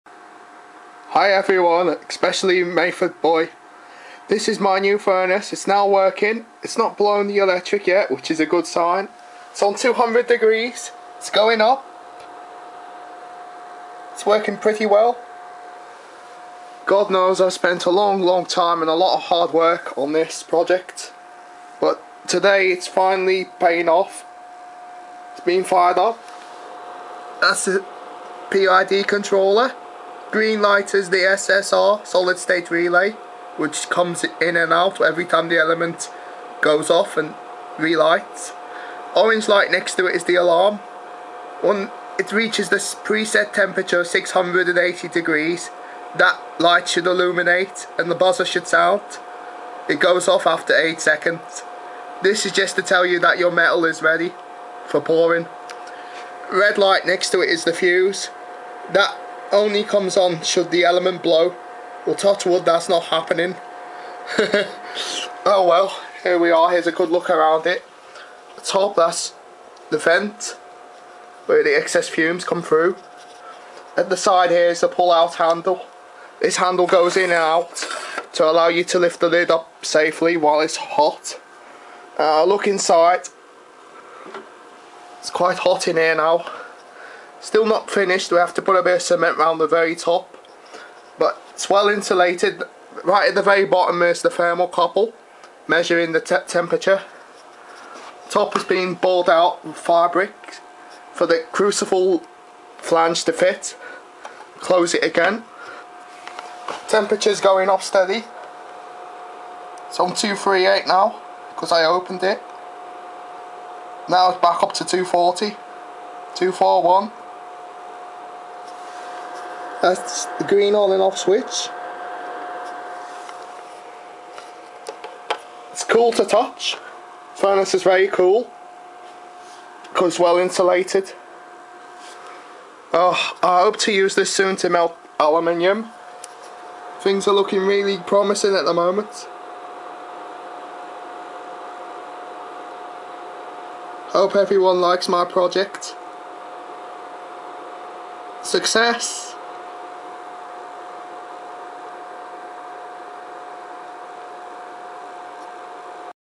My Electric Melting Furnace
My electric melting furnace project :o)

4 months later i have almost finished this project!

Features are 2000 watt 240v ac coil wound Kanthal A1 element, intelligent 7 programme JLD 612 Pid controller with alarm for when preset temperature is reached, large steel crucible high capacity crucible (5kg liquid capacity), fused and protected muffle element, 2 internal cooling fans to keep the electrics cool during operation, pull out handle for safely lifting/opening the furnace lid, indicator  lamps to show various functions of operation, double insulated with 28 grade fire bricks and 13mm ceramic insulation wool, Compact dimensions and generous melting capacity! - at long last a proper compact furnace!